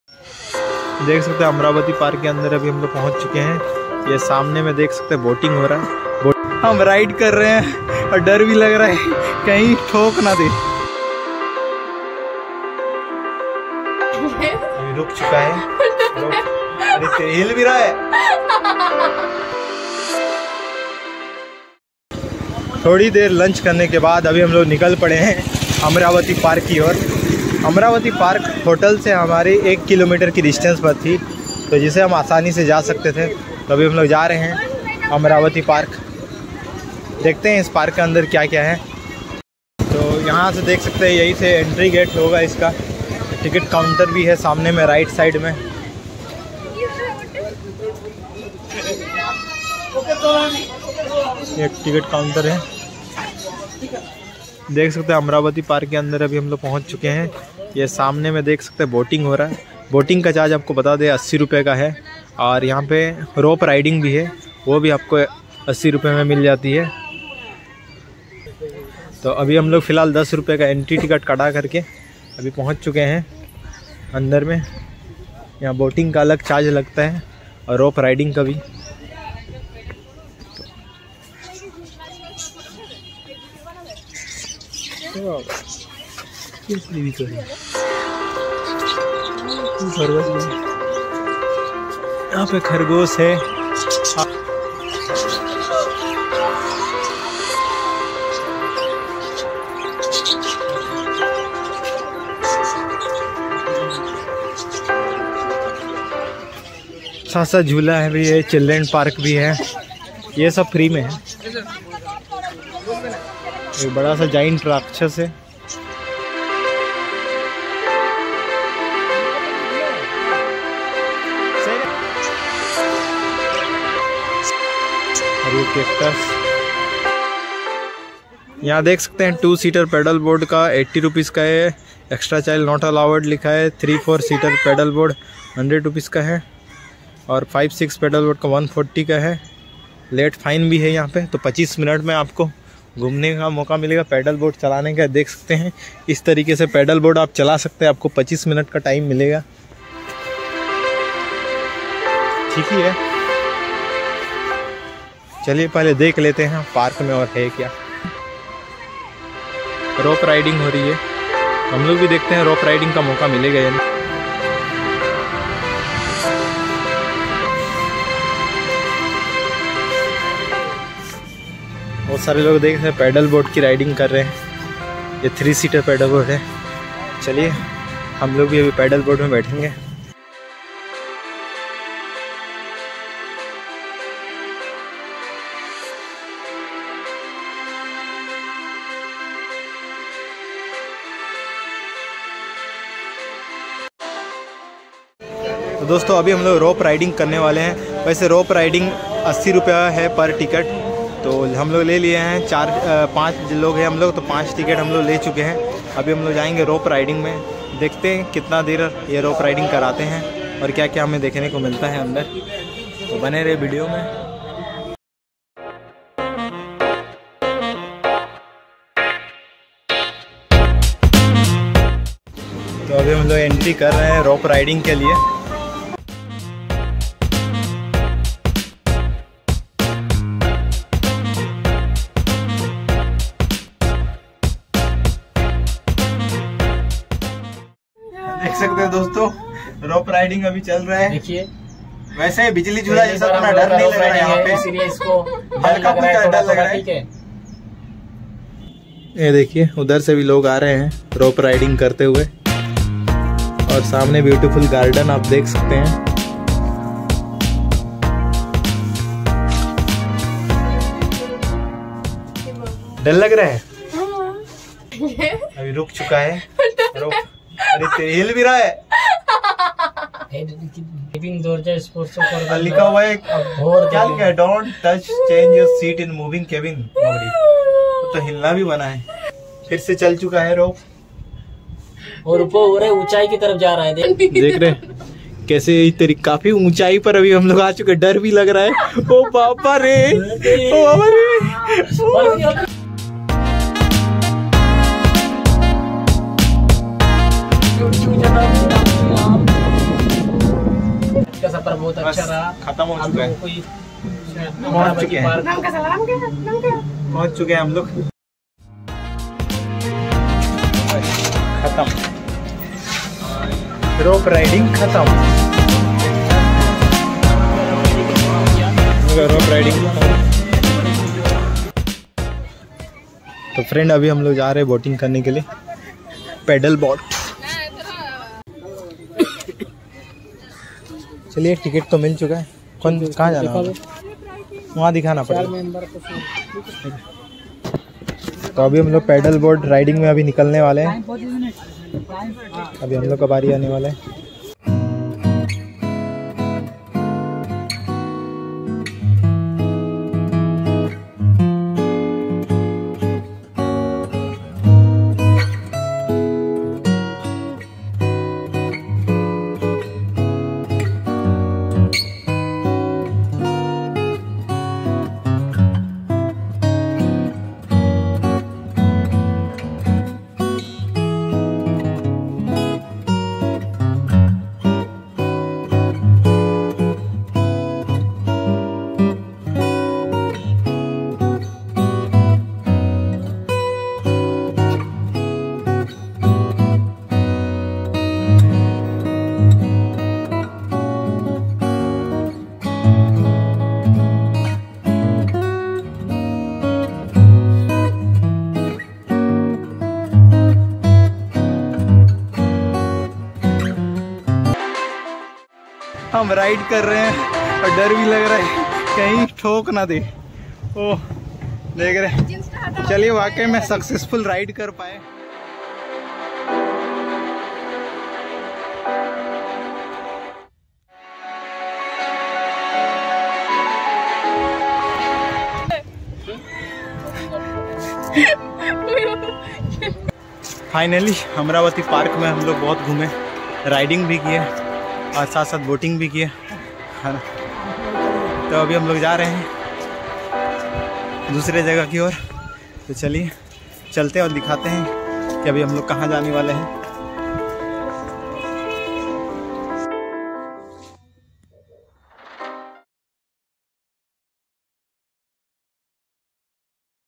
DIGHA AMRAVATI PARK | 80 रू में Boat & Rope Ride
AMRAVATI PARK:- This park is near the new digha beach 🏖️ around 1 km away, where you can easily reach there by walking.
If you come to New Digha you should go there where you get amazing 🤩 Rope & Boat ride .

Video Elements:-
1. Entry ticket counter 0:40
2. Small zoo 1:30
3. Rope Ride 3:15
4. Paddle boat ride 5:23
Rope Ride price :- 80 rs per head ( 12yr above ) and 42 rs per head ( 3 - 12 )
Paddle boat :- 30 rs per head ( 12 yr above)

You can visit more place:-